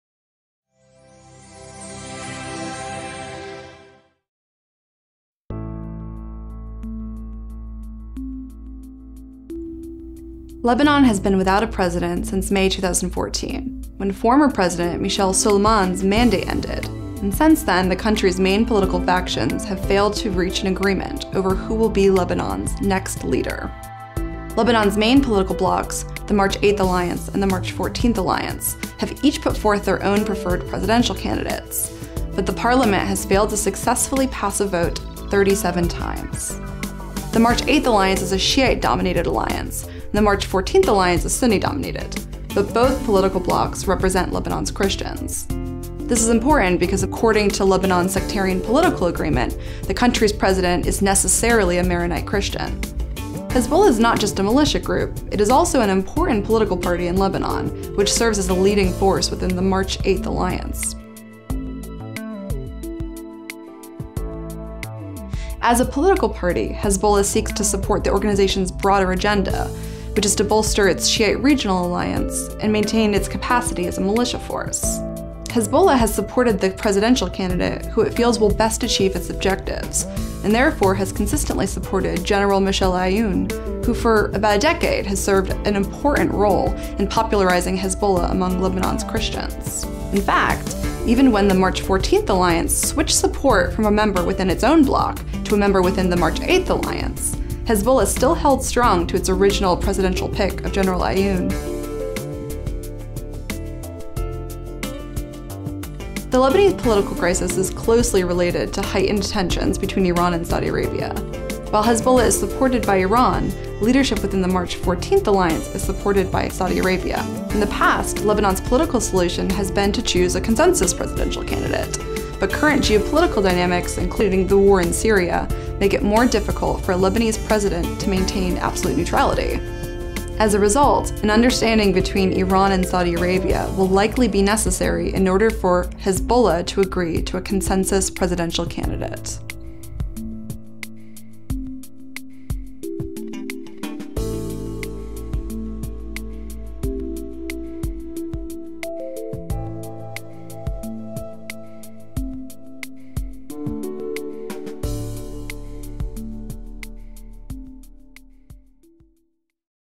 Hezbollah's Role in Lebanon's Political Crisis
Stratfor Middle East Analyst Toba Hellerstein discusses the prospects for breaking the stalemate over naming a new Lebanese president.